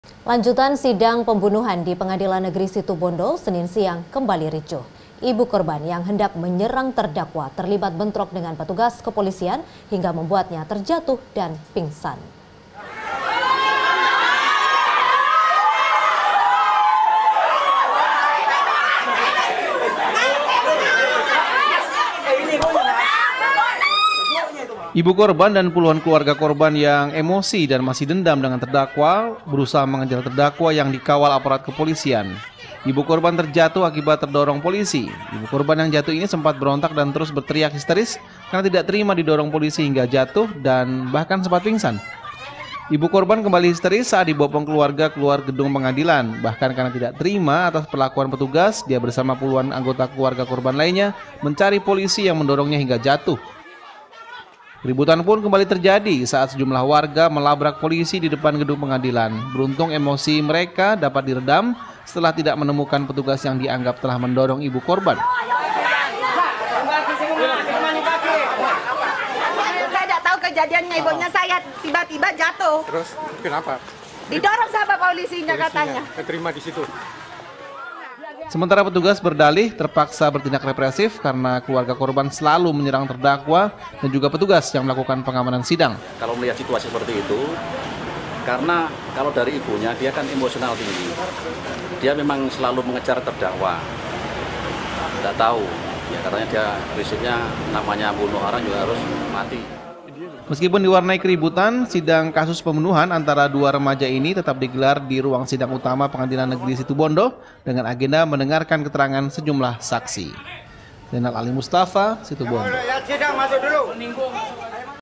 Sidang Ricuh, Ibu & Keluarga Korban Kejar Terdakwa
Lanjutan sidang pembunuhan di pengadilan negeri situbondo , berujung ricuh. Ibu korban yang hendak menyerang pelaku , terlibat bentrok dengan petugas kepolisian, hingga membuatnya terjatuh dan histeris . Puluhan keluarga yang tidak terima pun , langsung menantang polisi , dan memaki hingga memicu keributan di luar gedung sidang.